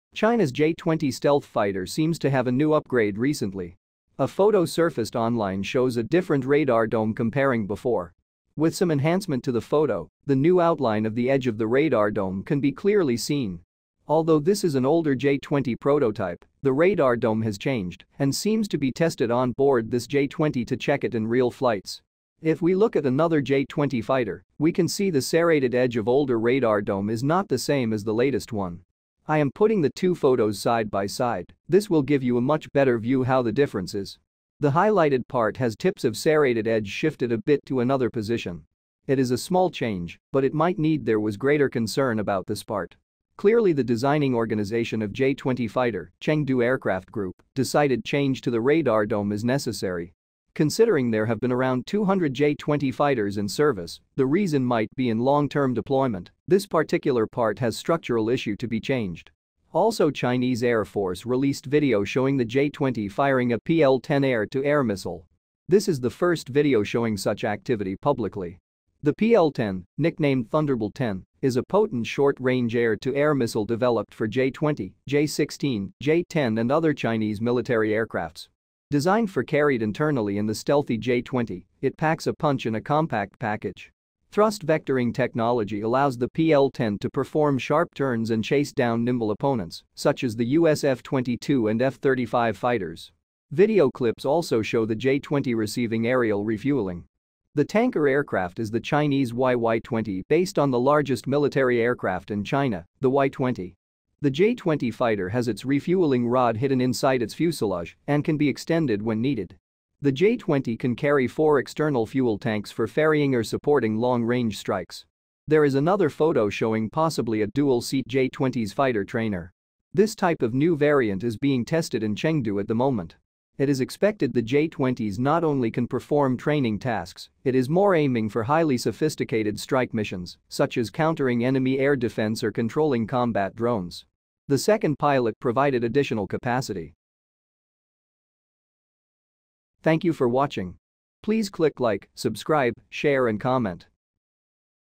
J-20 is changed: Design flaw fixed or an enhancement for this Chinese stealth fighter?
China’s J-20 stealth fighter seems to have a new upgrade recently. A photo surfaced online shows a different radar dome comparing before. With some enhancement to the photo, the new outline of the edge of the radar dome can be clearly seen. Although this is an older J-20 prototype, the radar dome has changed and seems to be tested onboard this J-20 to check it in real flights.

If we look at another J-20 fighter, we can see the serrated edge of older radar dome is not the same as the latest one.

I am putting the two photos side by side, this will give you a much better view how the difference is. The highlighted part has tips of serrated edge shifted a bit to another position. It is a small change but it might need there was greater concern about this part.

Clearly the designing organisation of J-20 fighter, Chengdu Aircraft group decided change to the radar dome is necessary. Considering there have been around 200 J-20 fighters in service, the reason might be in long term deployment, this particular part has structural issue to be changed.

Also Chinese Air Force released video showing the J-20 firing a PL-10 air to air missile. This is the first video showing such activity publicly. The PL-10, nicknamed "Thunderbolt-10", is a potent short-range air-to-air missile developed for J-20, J-16, J-10 and other Chinese military aircrafts. Designed for carried internally in the stealthy J-20, it packs a punch in a compact package. Thrust-vectoring technology allows the PL-10 to perform sharp turns and chase down nimble opponents, such as the U.S. F-22 and F-35 fighters.

Video clips also show the J-20 receiving aerial refueling. The tanker aircraft is the Chinese YY-20 based on the largest military aircraft in China, the Y-20. The J-20 fighter has its refueling rod hidden inside its fuselage and can be extended when needed.

The J-20 can carry four external fuel tanks for ferrying or supporting long range strikes.

There is another photo showing possibly a dual-seat J-20S fighter trainer. This type of new variant is being tested in Chengdu at the moment. It is expected the J-20S not only can perform training tasks, it is more aiming for highly sophisticated strike missions such as countering enemy air defense or controlling combat drones. The second pilot provided additional capacity.

Photo and video: Wiki common, Yangshipin site, Yangshijunshi weibo, PLADaily weibo, Zhongguokongjun wechat, Loveswift weibo